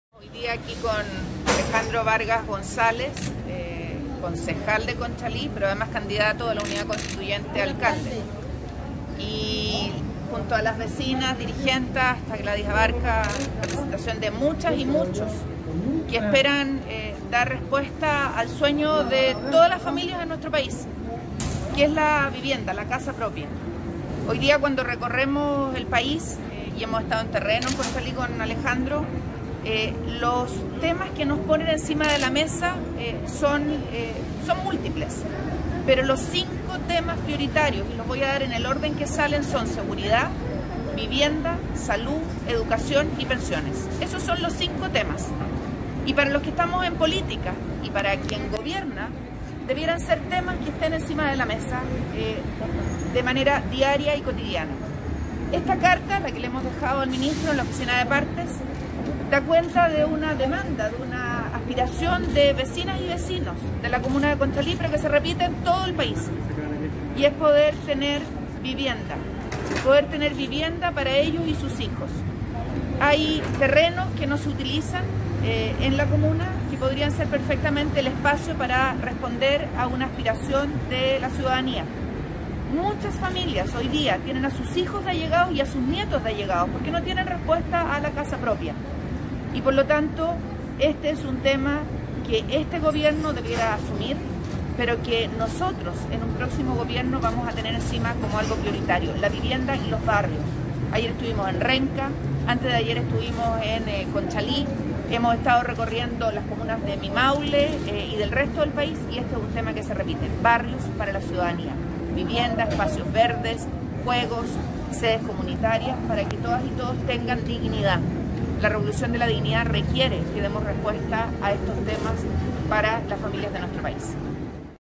Ximena Rincón y candidato Alcalde Alejandro Vargas, entregamos carta al Minvu
Junto al concejal y candidato alcalde la Comuna, Alejandro Vergas y vecinos de Conchalí, pedimos al Ministerio de Vivienda, celeridad en entrega de terrenos del Ejército para desarrollar proyectos de ViviendasSociales y AreasVerdes
Hay más de dos mil terrenos en todo Chile y su cesión ayudará a paliar déficit habitacional